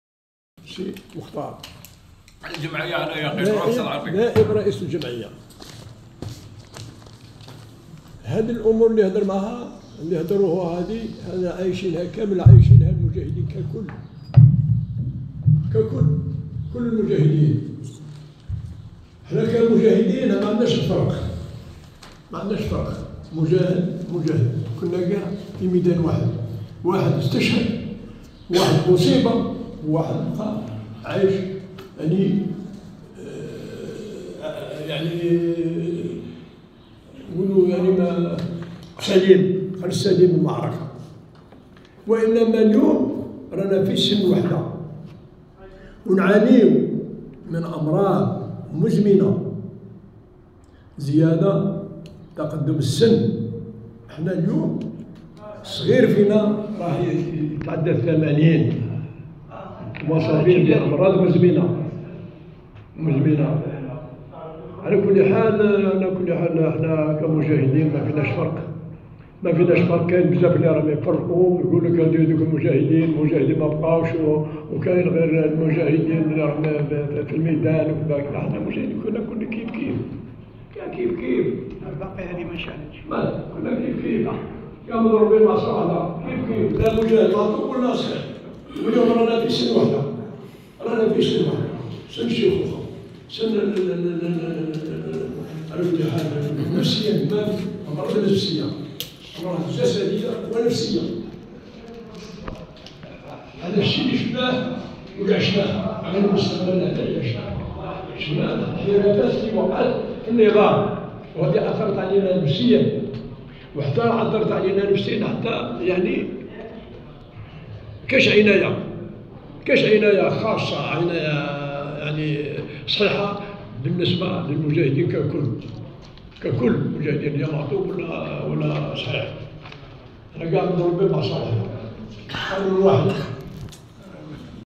الملتقى الوطني لجمعية الوطنية لكبار معطوبي حرب التحرير بولاية وهران 12/06/2023
الملتقى الوطني بمناسبة إ حياء الذكرى ال 61 لعيدي  الاستقلال و الشباب